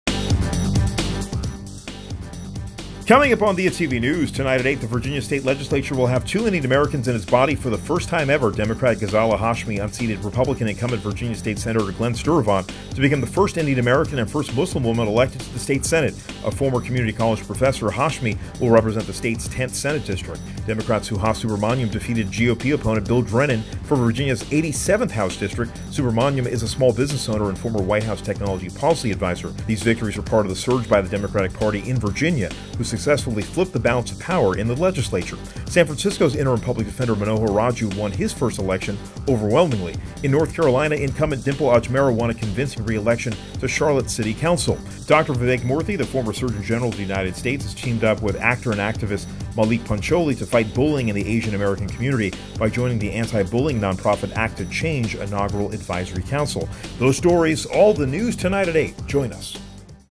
Indian Americans Hashmi, Subramanyam Elected To Va Legislature | Diya TV News
Indian Americans Hashmi, Subramanyam Elected To Va Legislature

The Virginia state legislature will have two Indian Americans in its body for the first time ever.
Democrat Ghazala Hashmi unseated Republican incumbent Virginia state Senator Glen Sturtevant to become the first Indian American and first Muslim woman elected to the state Senate. A former community college professor, Hashmi will represent the state’s 10th Senate District.
Democrat Suhas Subramanyam defeated GOP opponent Bill Drennan for Virginia’s 87th House District. Subramanyam is a small business owner and former White House technology policy advisor to President Obama.
These victories were part of a surge by the Democratic Party in Virginia, who successfully flipped the balance of power in the legislature.

Manohar Raju Elected SanFrancisco Public Defender

San Francisco’s interim public defender Manohar Raju won his first election overwhelmingly.

Dimple Ajmera Reelected To Charlotte City Council

In North Carolina, incumbent Dimple Ajmera won a convincing re-election to Charlotte’s City Council.

Former Surgeon General Vivek Murthy Joins Nonprofit Act To Change

The 19th Surgeon General of the United States and Wellness Advocate Dr. Vivek Murthy has teamed up with actor and activist Maulik Pancholy to fight bullying in the Asian American Community by joining the anti-bullying nonprofit Act To Change inaugural Advisory Council.

Diya TV ®
Enlighten. Entertain. Inspire.™